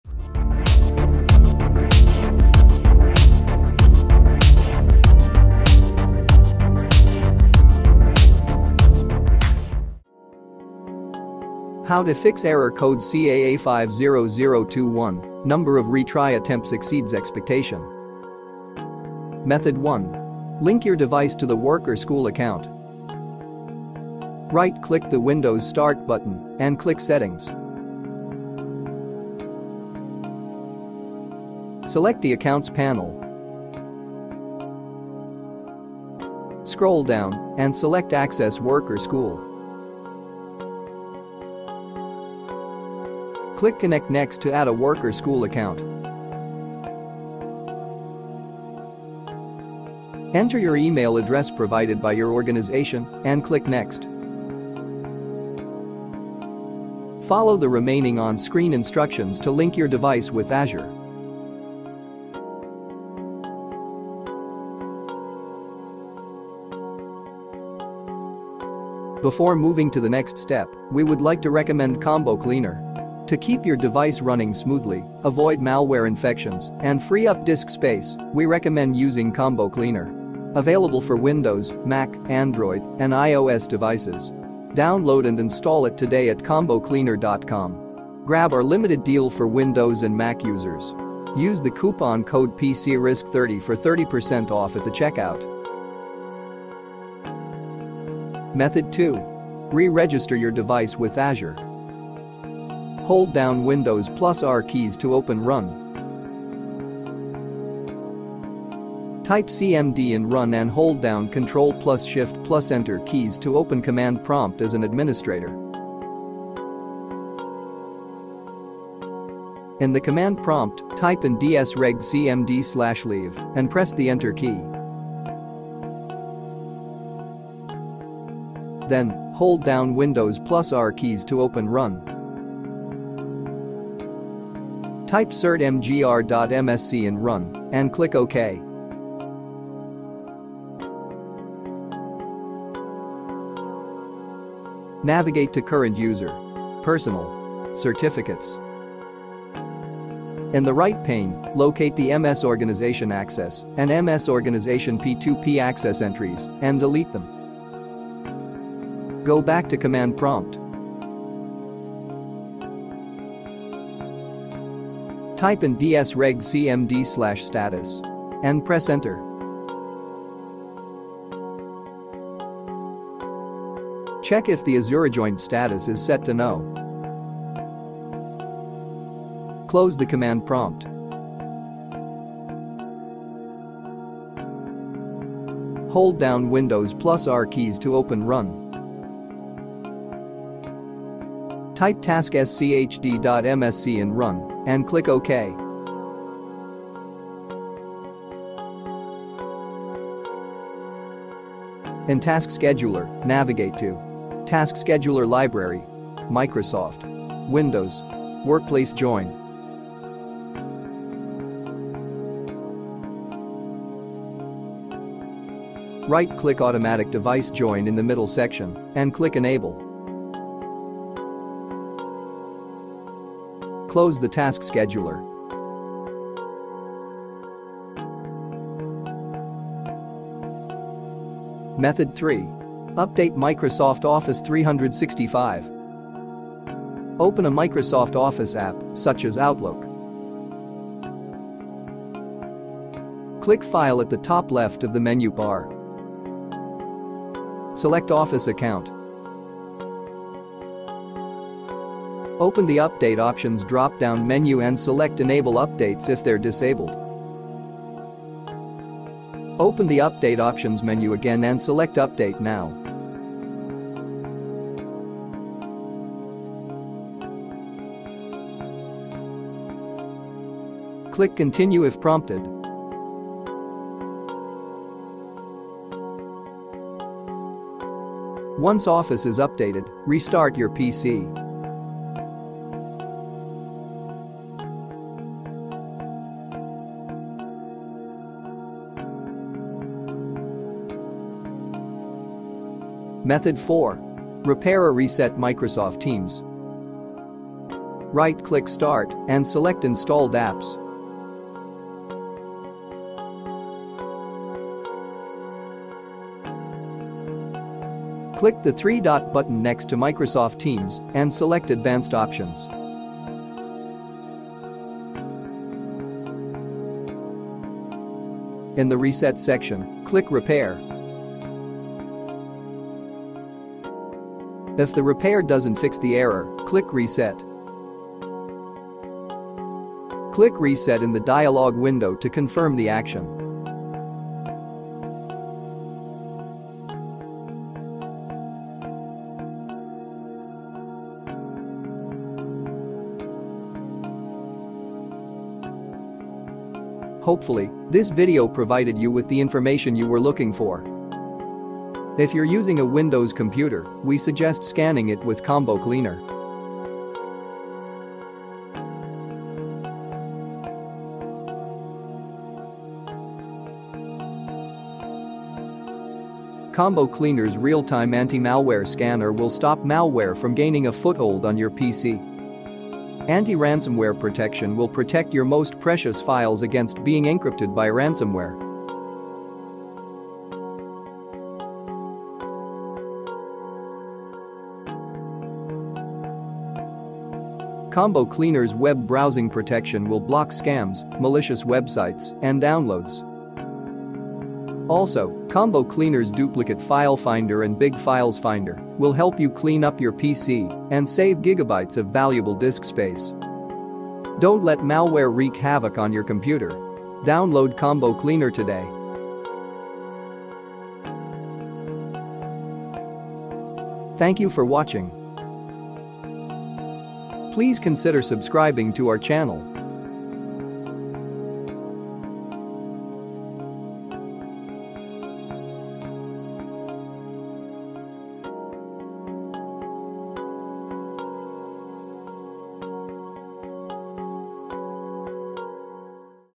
How to Fix Error Code CAA50021, Number of Retry Attempts Exceeds Expectation?
Video showing how to fix Error Code CAA50021, Number of Retry Attempts Exceeds Expectation.